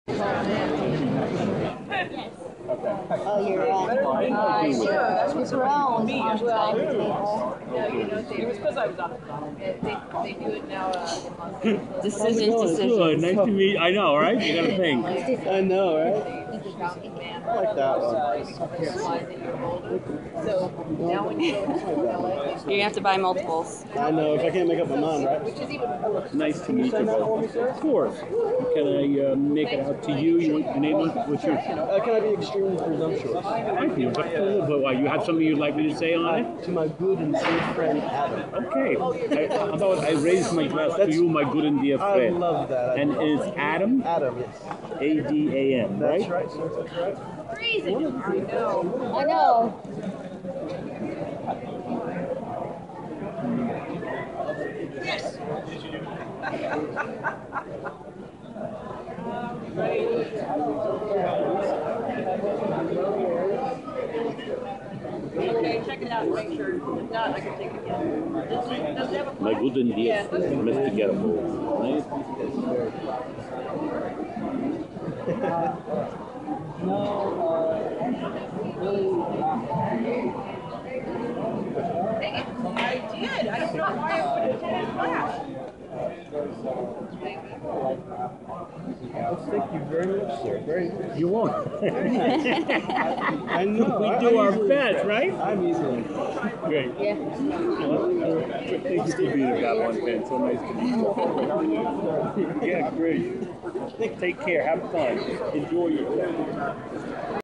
Peter Jurasik Autographing at Space City Con 2014
Peter Jurasik autographing my photo at Space City Con 2014 in Galveston, TX.